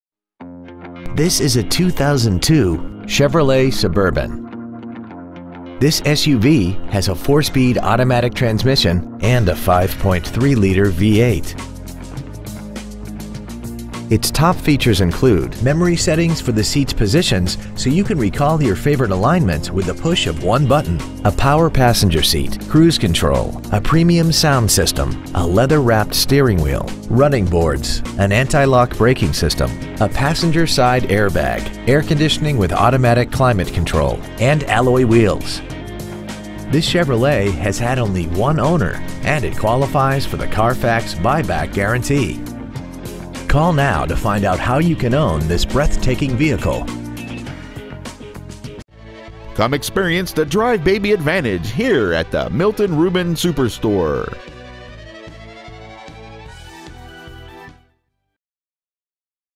2002 Chevrolet Suburban Hephzibah GA
Year : 2002
 Make : Chevrolet
 Model : Suburban
 Engine : 5.3L V8 FI
 Trans . : Automatic
 Exterior : Redfire Metallic
 Miles : 111,592
 Interior : Light Cashmere
 Stock : C204411

 Preowned 2002 Chevrolet Suburban Augusta GA